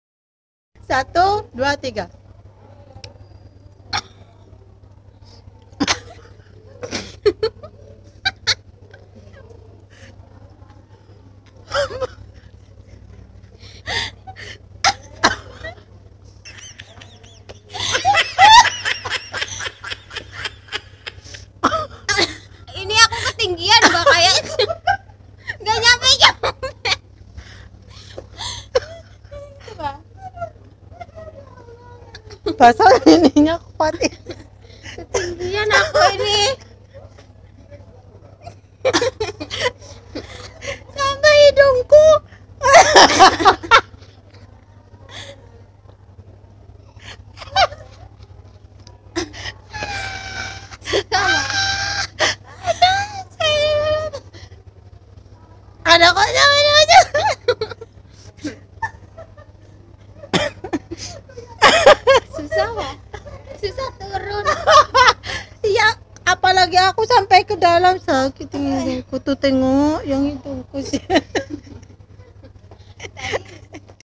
lomba viral persiapan agustusan ngakak bareng biar ngak stresss
persiapan agustusan
dokumentsi saya bersama teman teman untuk jadi kenangan ketik kita sudah tidak bersama sama lagi